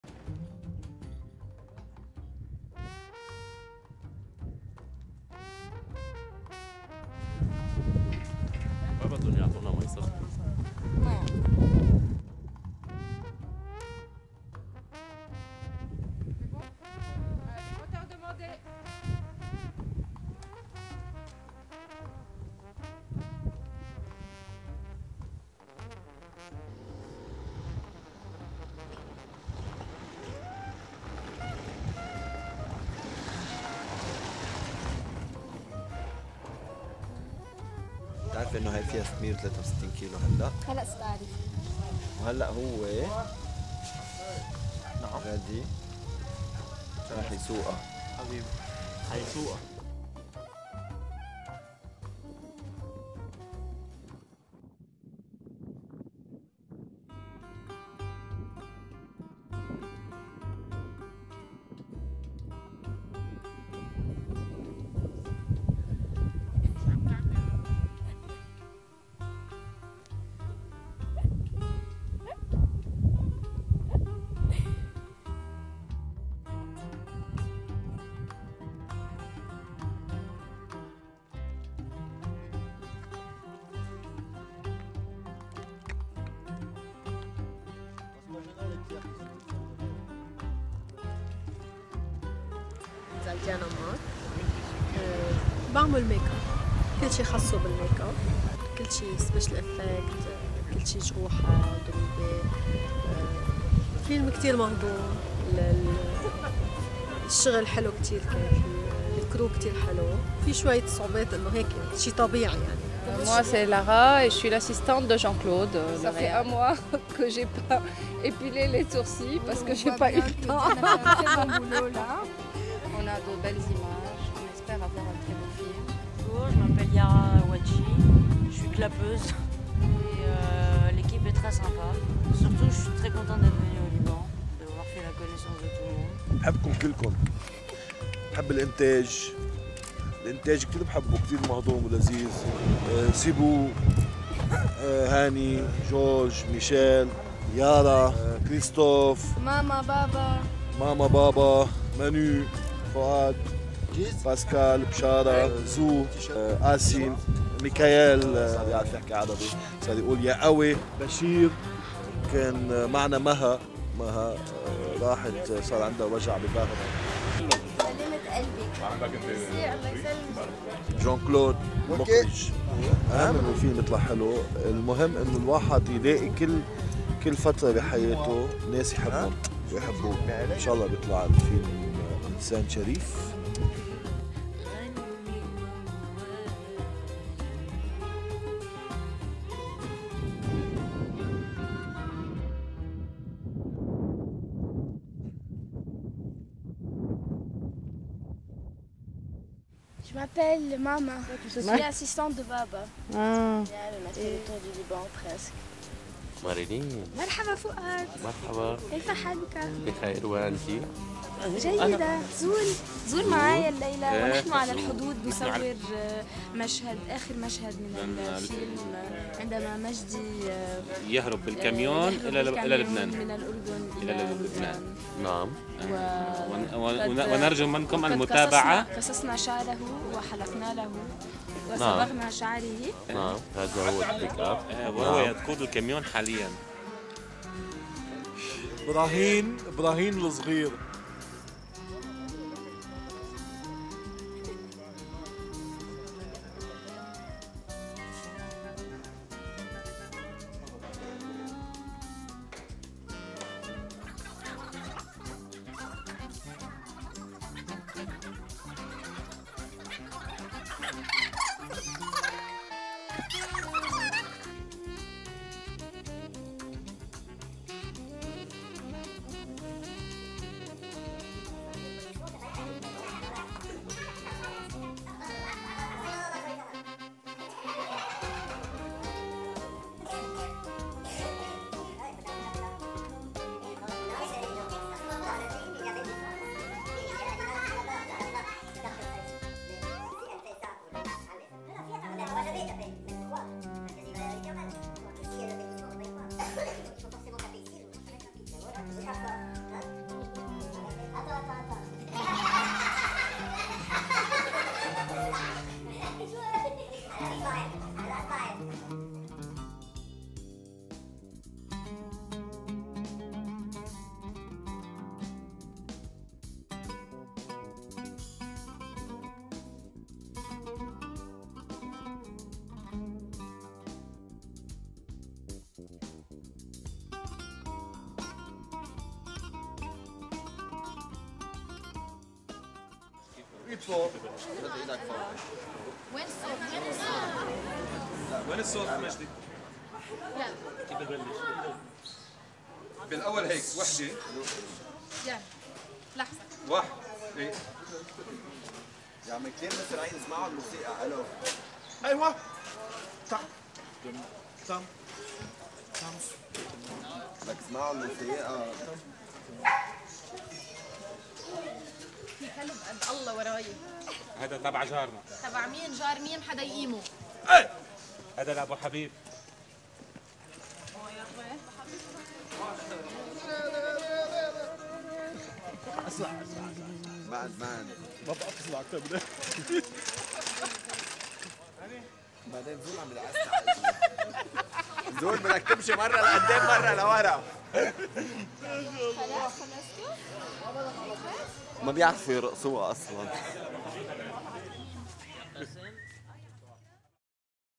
A MAN OF HONOR (2012) behind the scenes by Marilyn Farah , Part 2 of 2
A MAN OF HONOR (2012) is a Lebanese feature film directed by Jean-Claude Codsi and produced by Aflam Films production.

This is part 2 of 2 of behind the scenes, filmed and edited by Marilyn Farah.

A MAN OF HONOR Synopsis:
Brahim has a chance encounter with a mysterious woman he knew 20 years ago. Because of her, he committed a murder and now has to return home to face his tumultuous past.